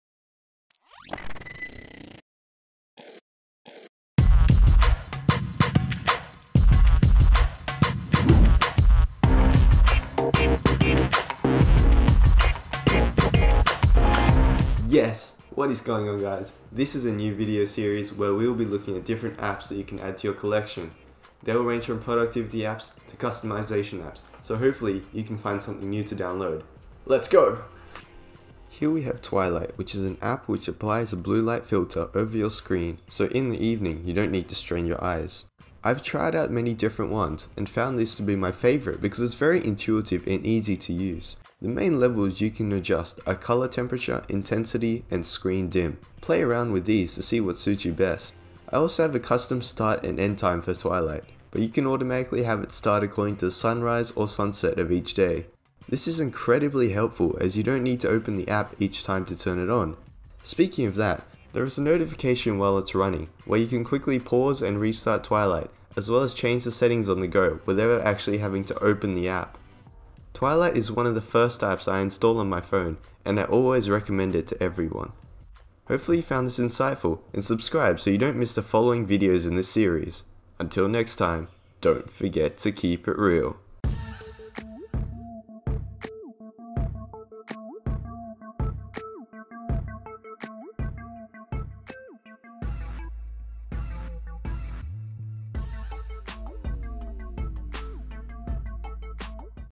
Twilight: Blue Light Filter - 1 Minute App Showcase
In this new series 1 Minute App Showcase, we'll take a look at different apps that you could potentially download and add to your Android. In this video we look at Twilight, a blue light filter app for your phone.